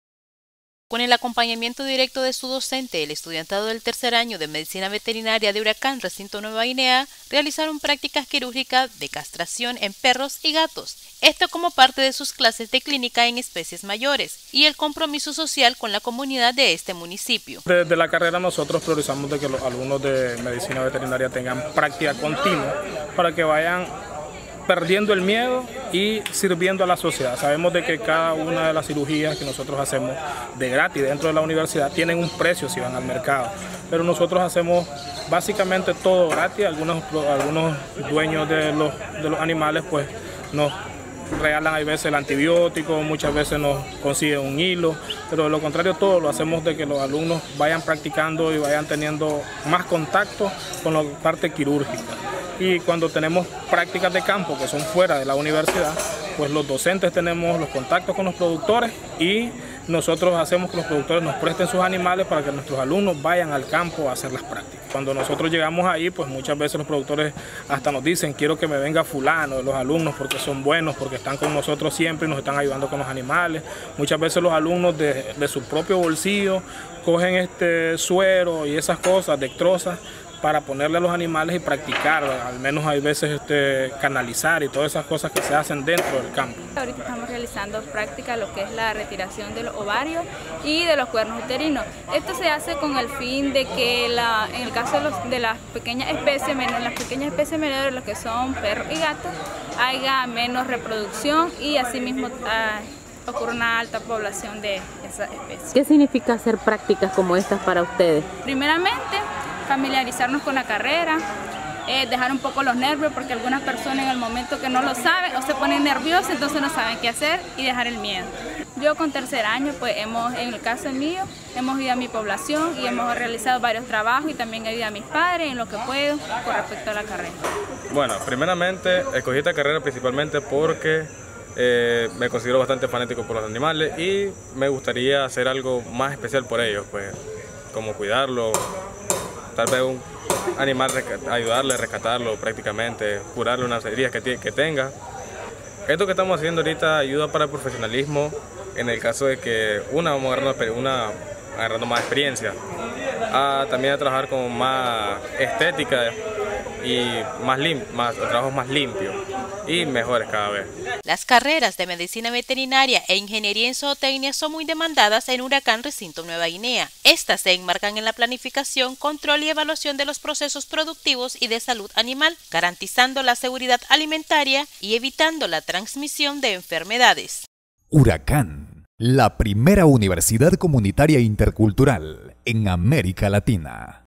𝐌𝐄𝐃𝐈𝐂𝐈𝐍𝐀 𝐕𝐄𝐓𝐄𝐑𝐈𝐍𝐀𝐑𝐈𝐀 I estudiantes realizarón prácticas quirúrgicas en caninos y felinos
Con el acompañamiento directo de su docente, el estudiantado del tercer año de Medicina Veterinaria de URACCAN recinto Nueva Guinea realizó prácticas quirúrgicas de castración en caninos y felinos, como parte de sus clases de Clínica en Especies Mayores y también por el compromiso social con la comunidad del Municipio.